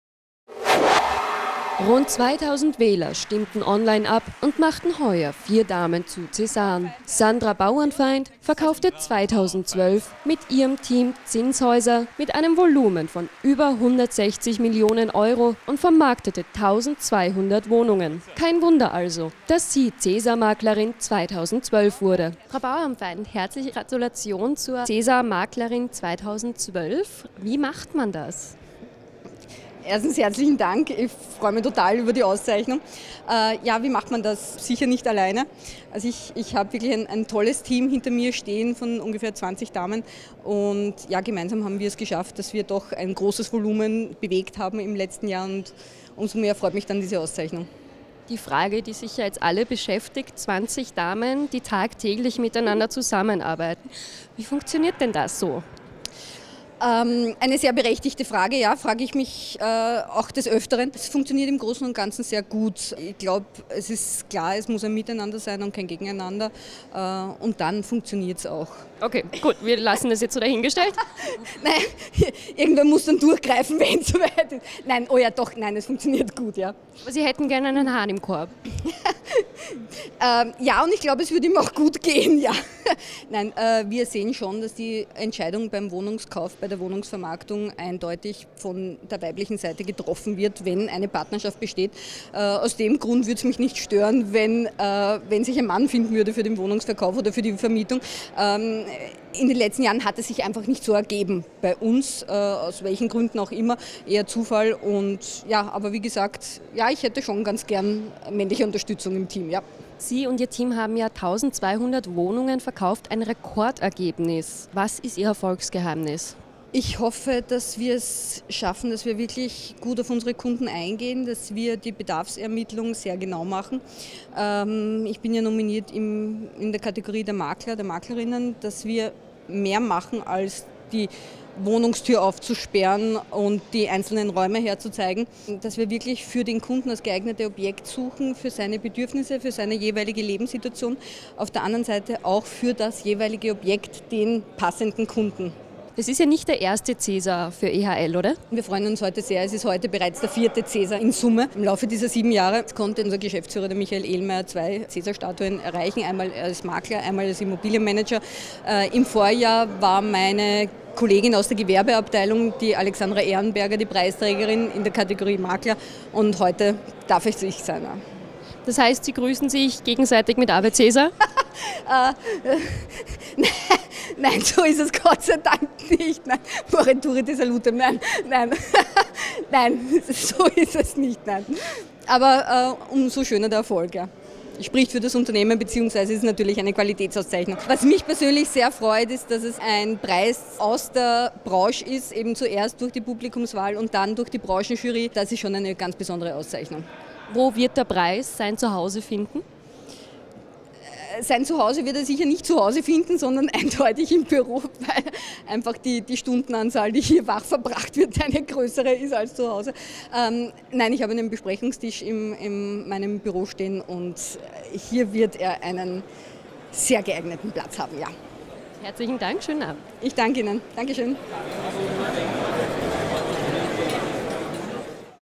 Sandra Bauernfeind wird Cäsar-Maklerin 2012
Vier Cäsaren  2012 gingen an das schöne -- und gleichzeitig starke -- Geschlecht. Eine der frischgebackenen Cäsarinnen ist Sandra Bauernfeind. Für die Leiterin des Bereichs Wohnimmobilien bei EHL und ihr 20-köpfiges Team war 2012 ein beeindruckendes Rekordjahr: Zinshäuser mit einem Volumen von über 160 Millionen Euro brachte Bauernfeind an den Mann. Außerdem wurden insgesamt 1200 Miet-, Vorsorge- und Eigentumswohnungen vermarktet und zusätzlich der Vorsorgewohnungsbereich ausgebaut. Somit ist EHL Marktführer. „Mir bedeutet der Preis sehr, sehr viel. Er ist eine Bestätigung für die Arbeit der letzten Jahre und wird noch dazu von unabhängigen Branchenkollegen vergeben. Das ist einfach wirklich eine Freude", strahlt Bauernfeind. Was ihr Erfolgsgeheimnis ist, sehen Sie jetzt im Interview!